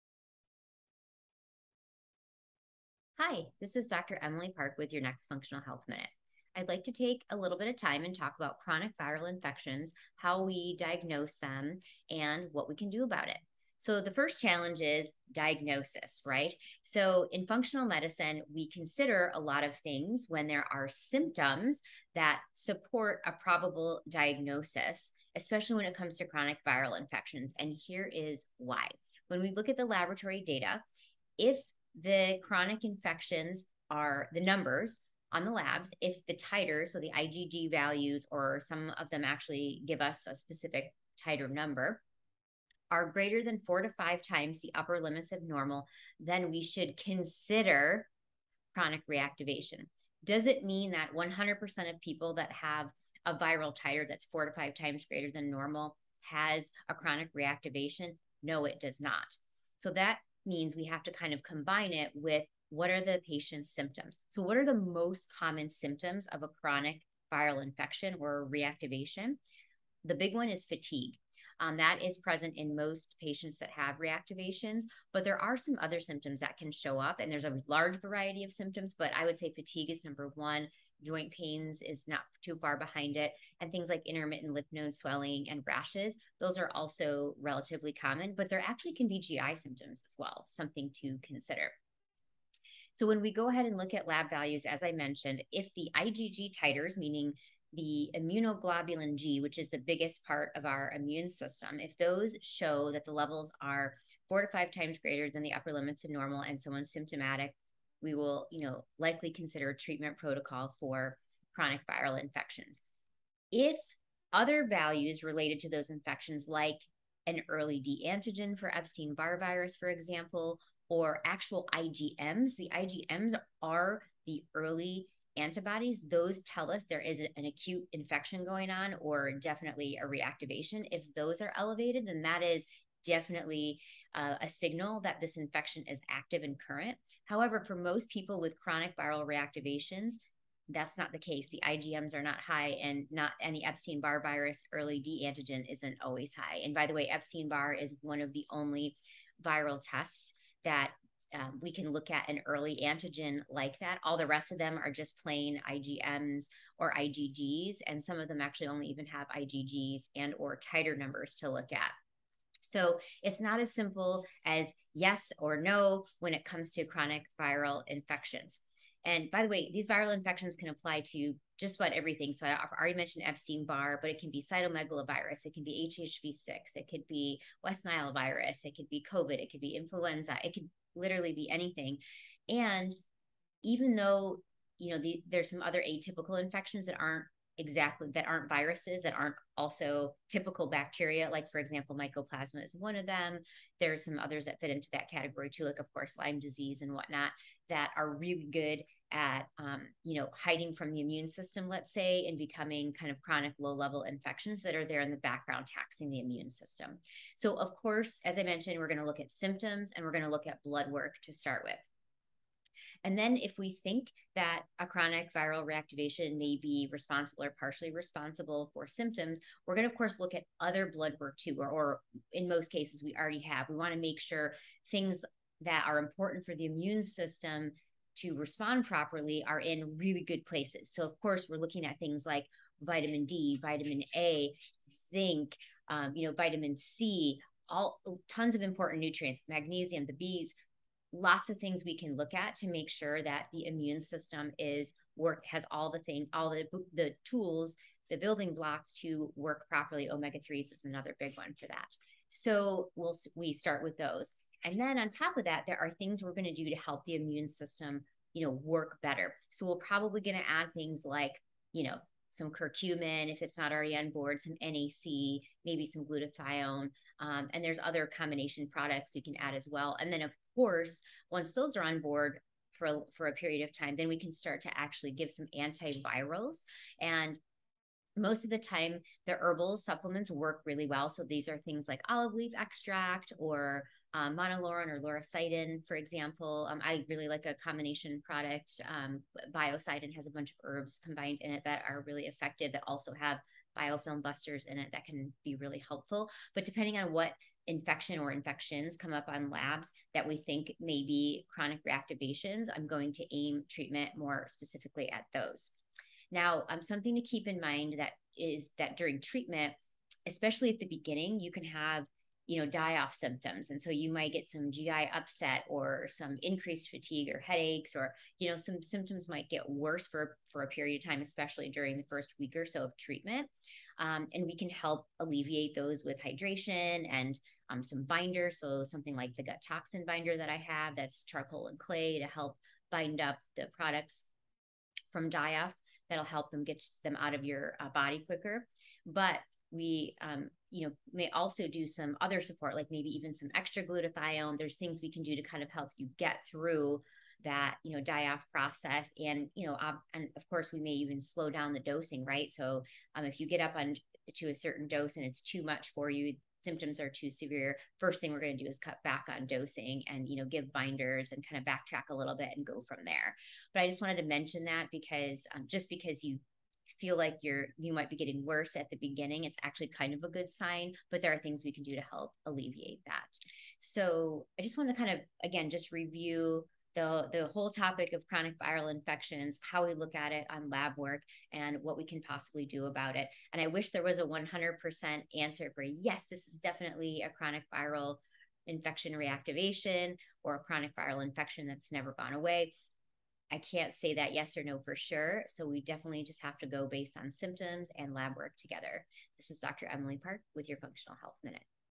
Chronic Viral Infections
In this Functional Health Minute, I'd like to take a little bit of time and talk about chronic viral infections, how we diagnose them and what we can do about it.